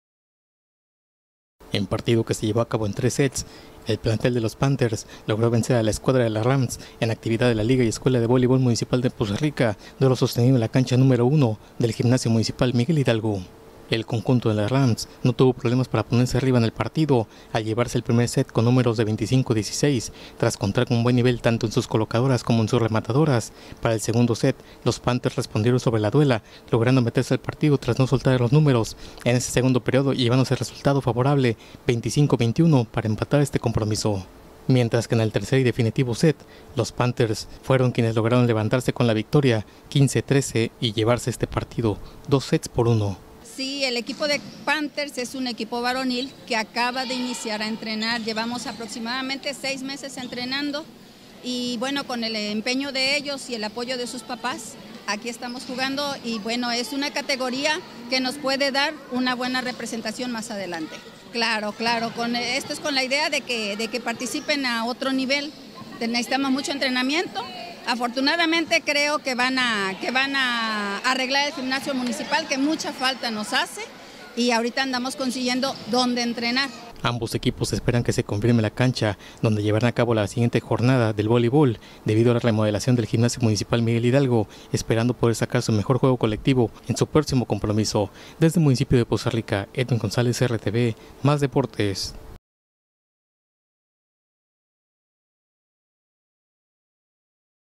PANTHERS SE IMPONE EN EL VOLEIBOL MUNICIPAL
El equipo de voleibol Panthers de Poza Rica vencen dos sets por uno a las ramz en partido llevado a cabo en el gimnasio municipal “Miguel Hidalgo”.
Reportero: Edwin González